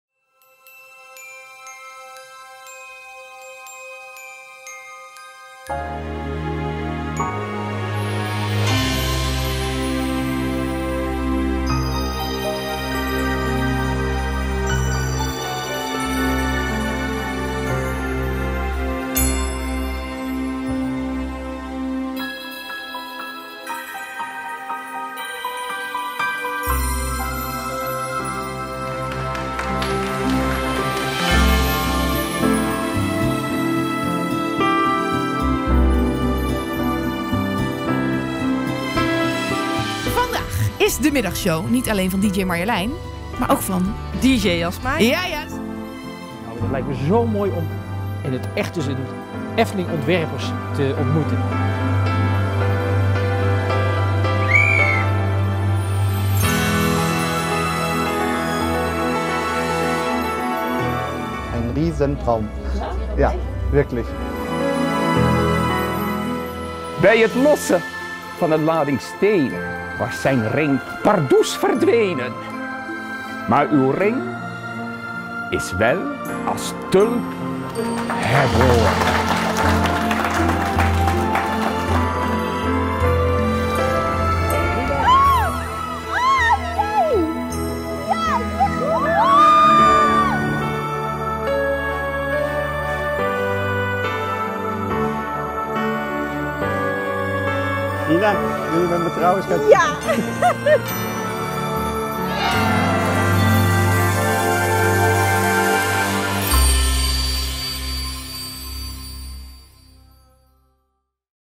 Efteling Wensboom: De meest bijzondere wensen gingen in vervulling! - Efteling
Tijdens de Winter Efteling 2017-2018 konden gasten hun mooiste wensen achterlaten bij de Efteling Wensboom in het park of via een formulier op de website.

Van een kijkje achter de schermen en een huwelijksaanzoek in een achtbaan tot een zoektocht naar een in 1960 verloren trouwring. In deze verzamel-video zie je welke wensen werkelijkheid werden!

Wil je meer zien en lezen over de wensen die in vervulling gingen? Kijk eens op

Kijk ook op of op voor het laatste nieuws over de Efteling.